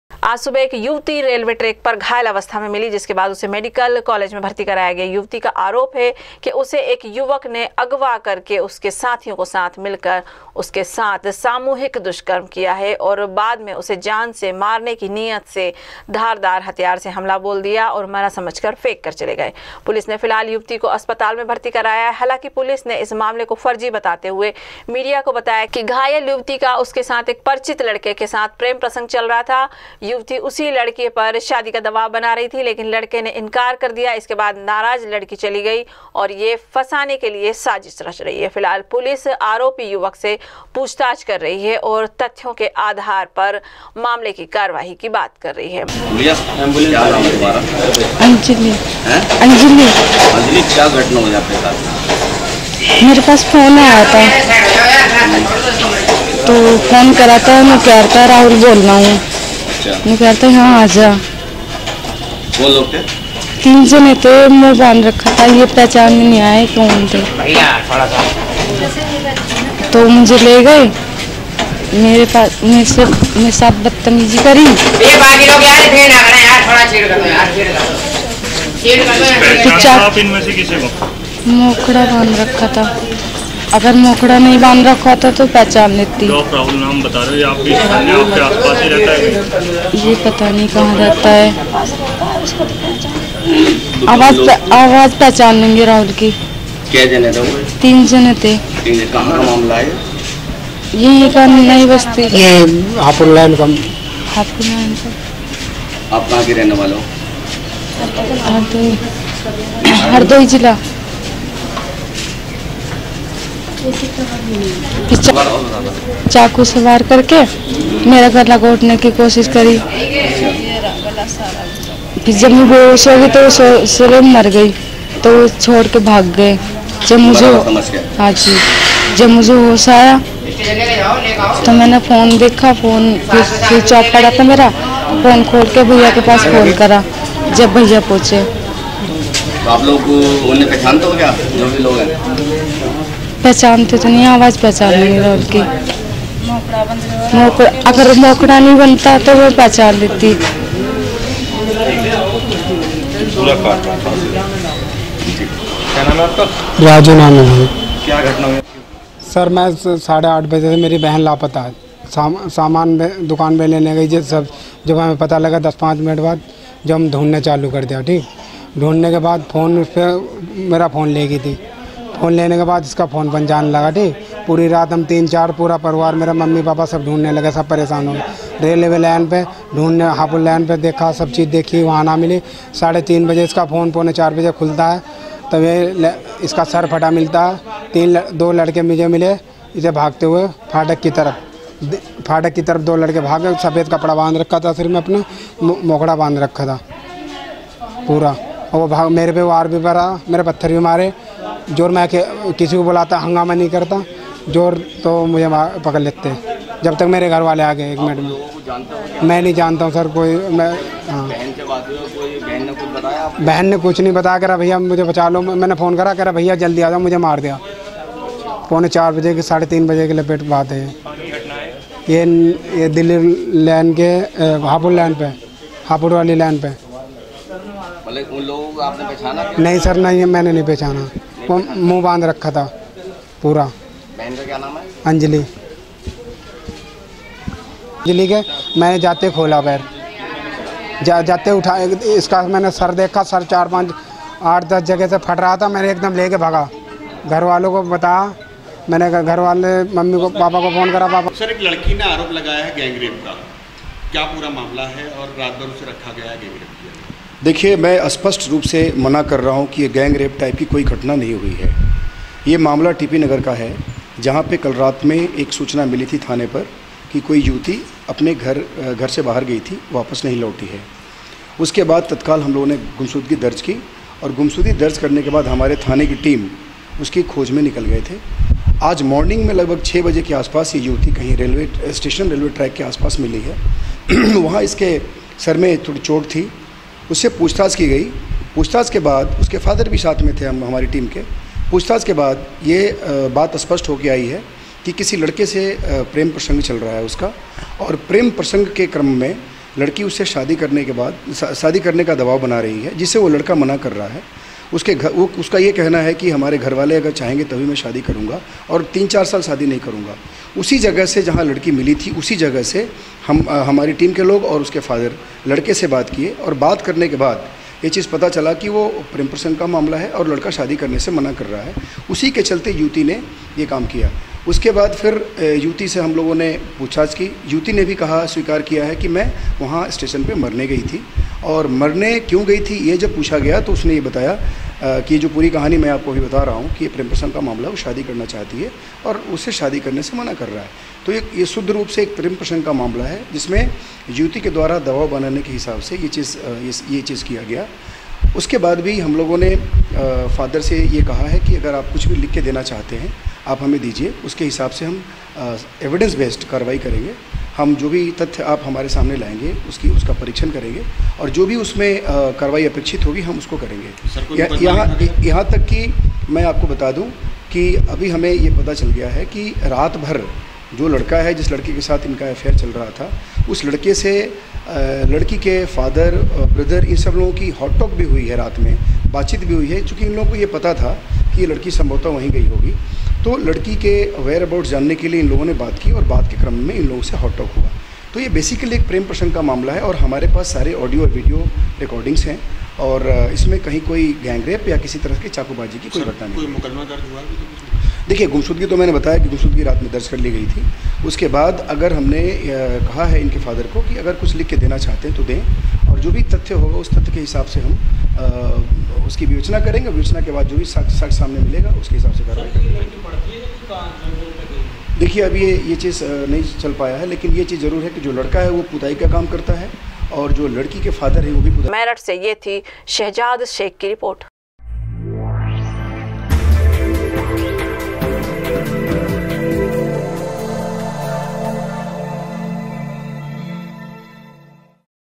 [ Meerut ] एक युवक ने युवती को अगवा कर अपने साथियों के साथ सामूहिक दुष्कर्म किया
आज सुबह एक युवती रेलवे ट्रैक पर घायल अवस्था में पड़ी मिली  जिसके बाद उसे मेडिकल कॉलेज में भर्ती कराया गया है युवती का आरोप है कि उसे एक युवक ने अगवा कर अपने साथियों उसके साथ सामूहिक दुष्कर्म किया और उसके जान से मारने की नियत से उसपर धारदार हथियार से हमला बोल दिया